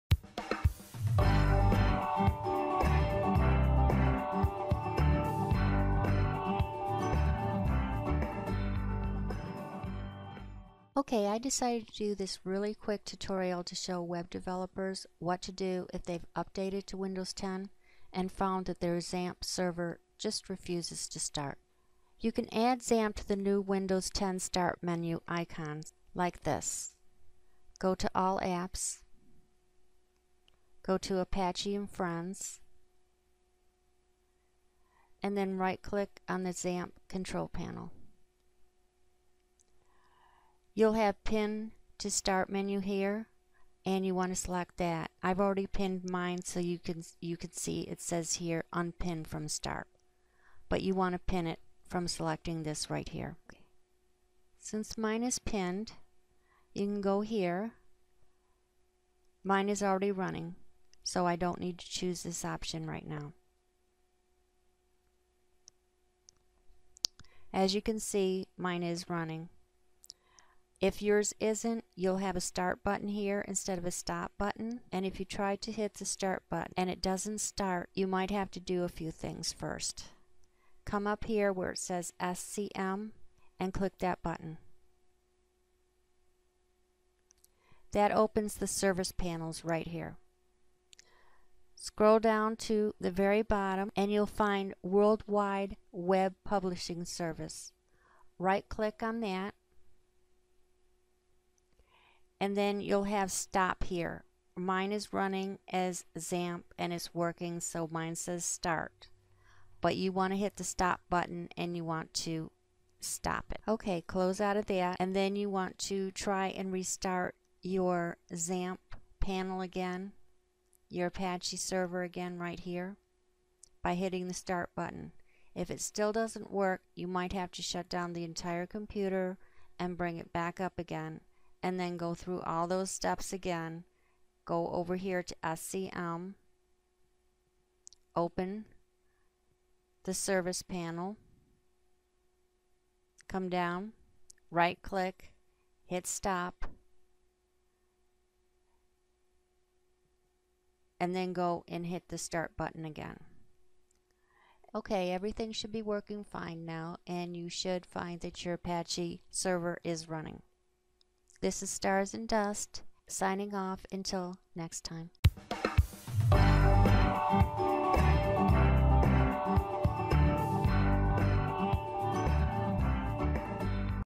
Xampp Not Starting in Windows 10
This tutorial describes how to be able to start the Xampp server after you have updated your system to Windows 10 and the server just doesn't seem to want to start.